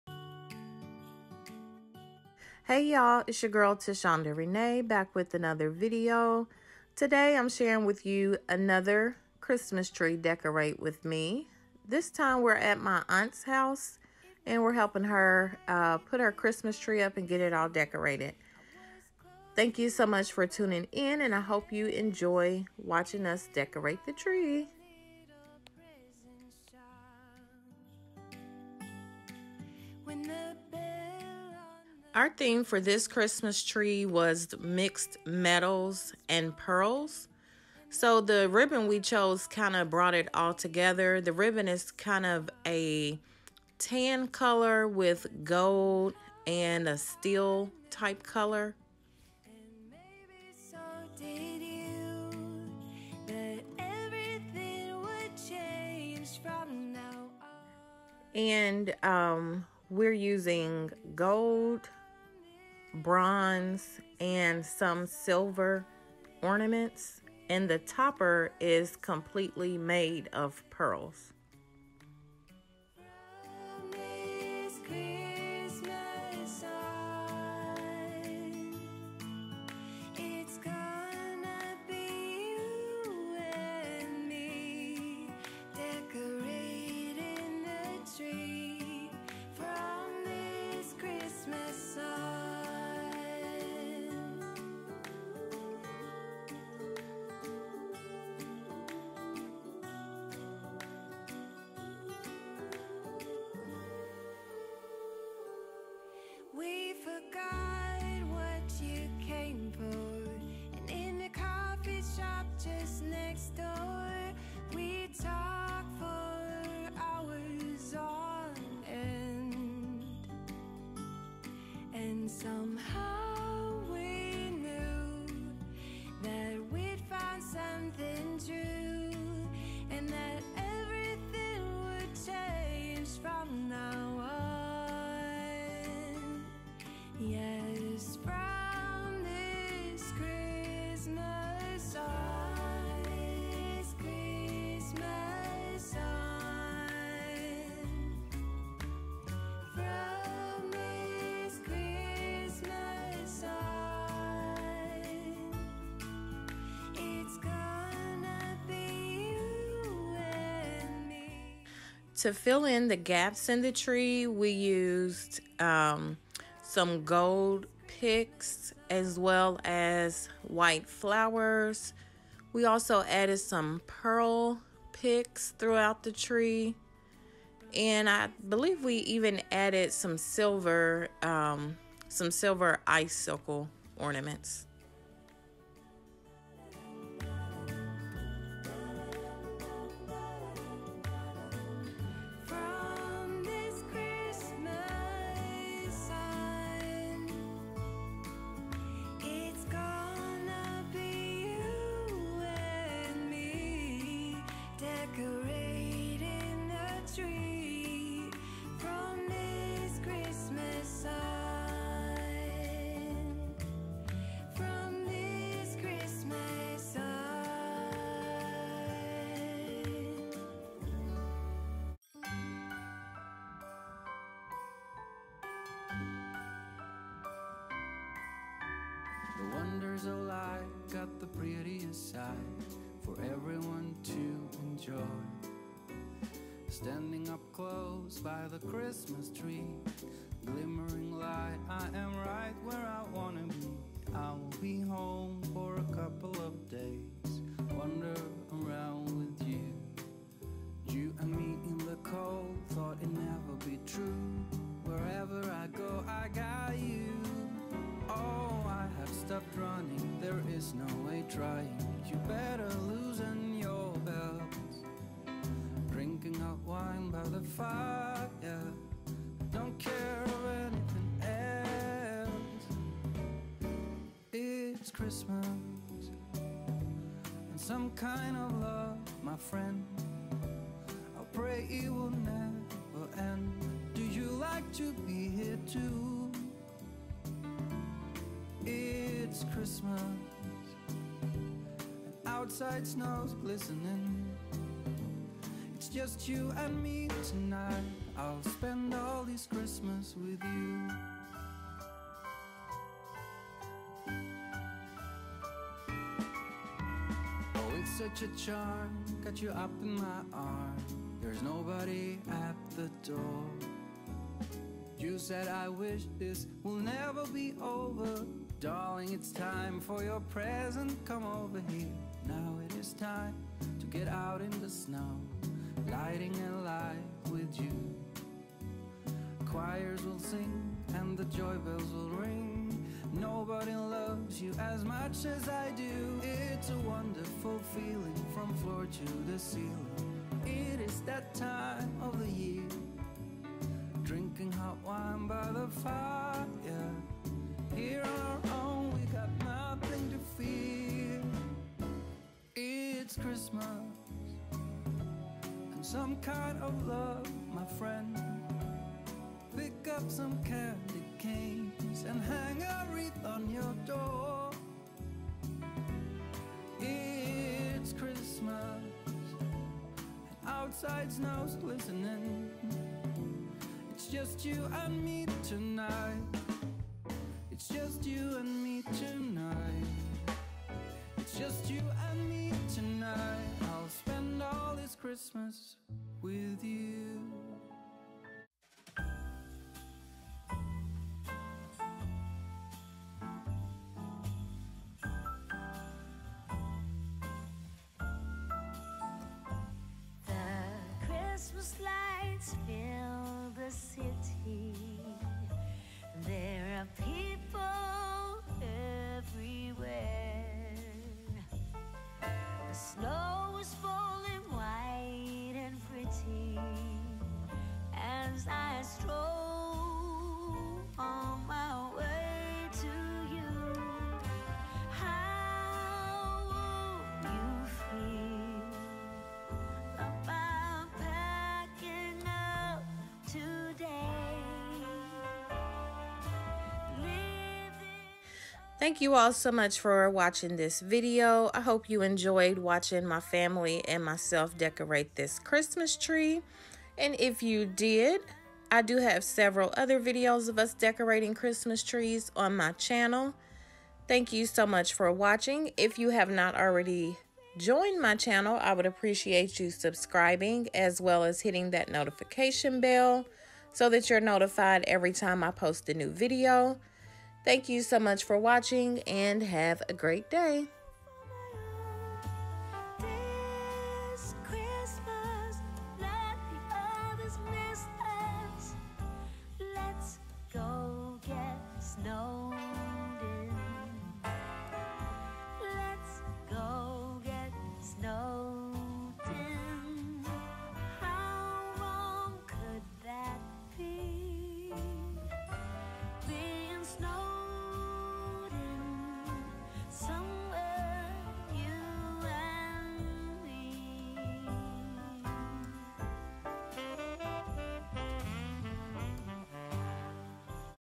Christmas Decorate With Me | 2020
Hey y'all! In today's video, watch me help my aunt decorate her Christmas Tree!!

Sony ZV-1:

Hey y’all! Today’s video is a decorate with me! My mom and I help my brother and sister in law decorate their Christmas tree!! I hope you enjoy!!

Tashondarenaeonyoutube